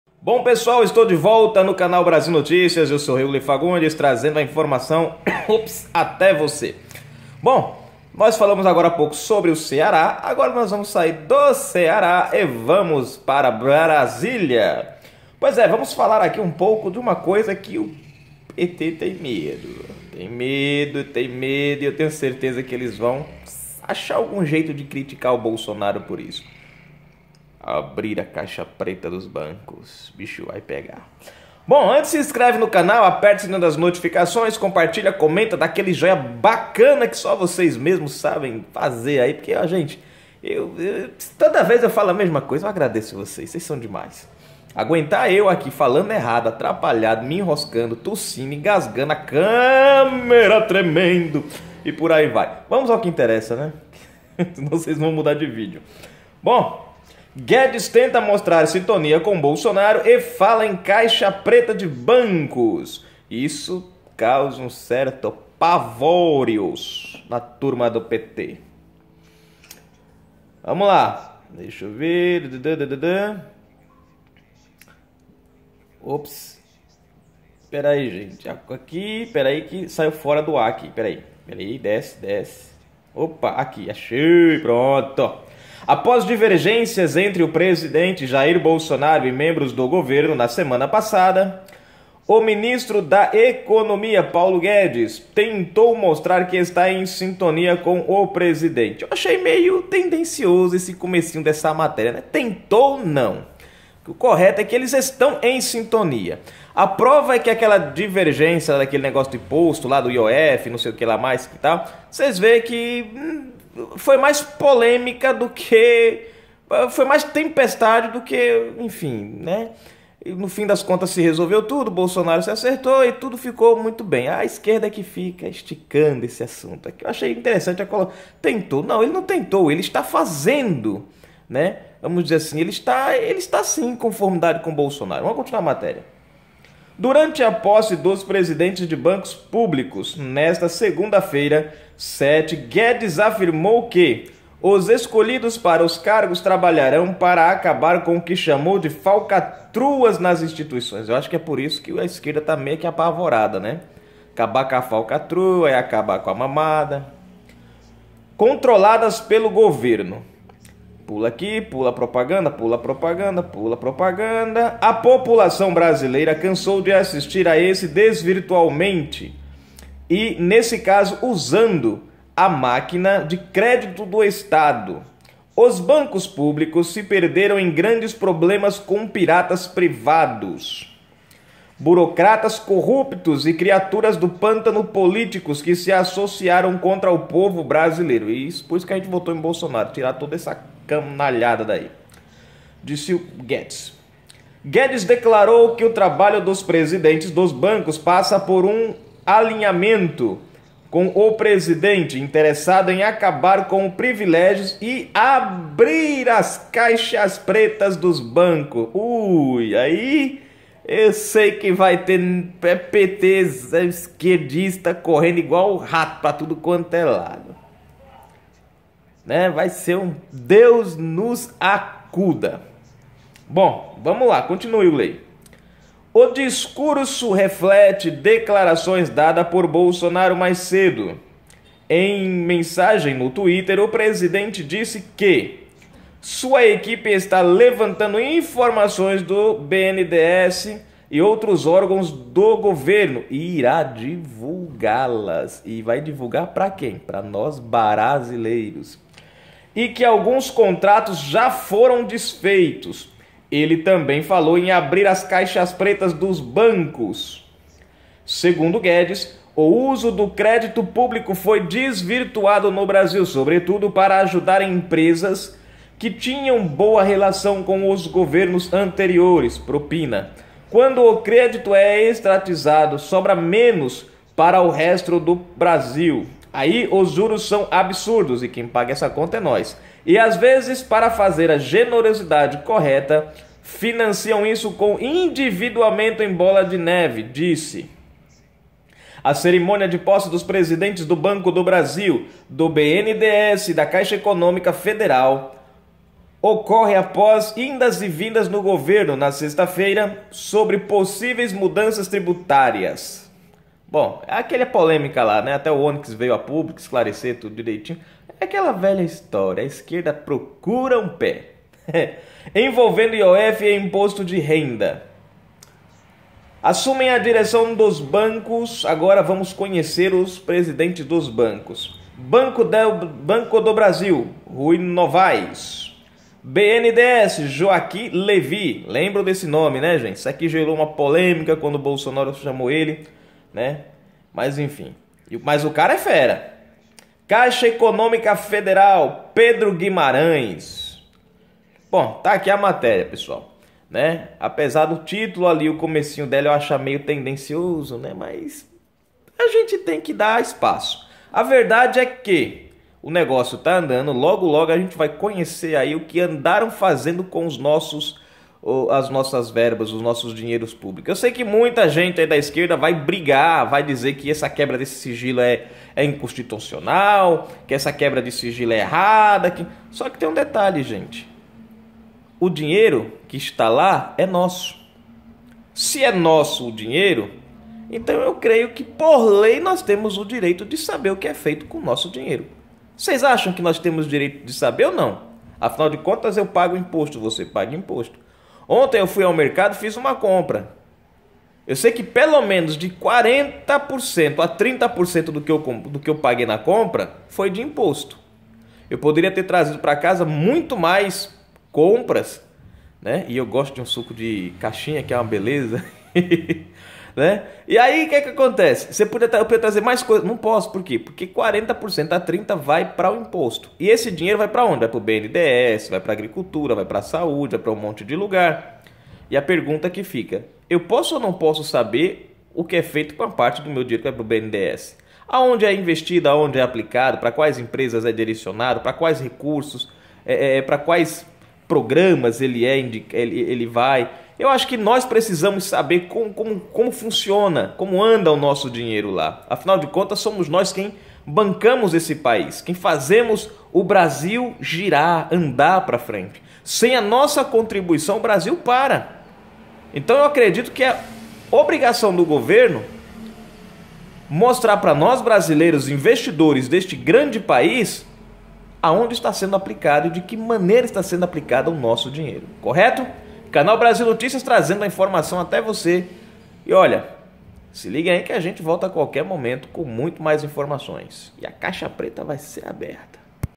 Para desespero do PT, Guedes fala em Abrir caixa preta dos bancos.
é agora que o PT entra em parafuso, Paulo Guedes anunciou que vai abrir a caixa preta dos bancos e revelar como foi usado o dinheiro público, na gestão petista.

Canal Brasil Noticias, trazendo a informação até você.